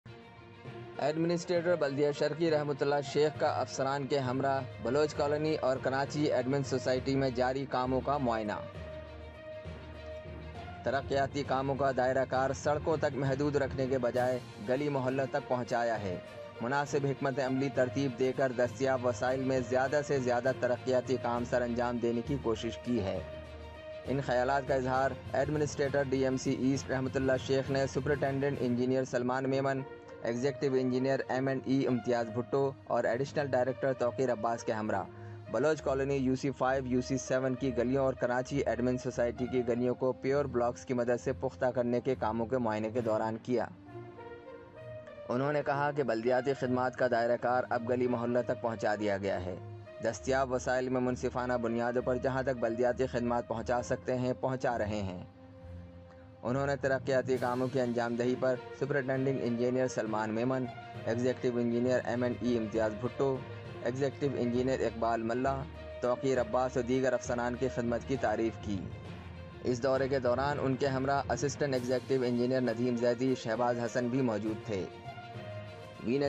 Administrator DMC East Rehmatullah Sheikh - visit street works
Administrator DMC East Rehmatullah Sheikh - visit baloch colony & khi admin society street works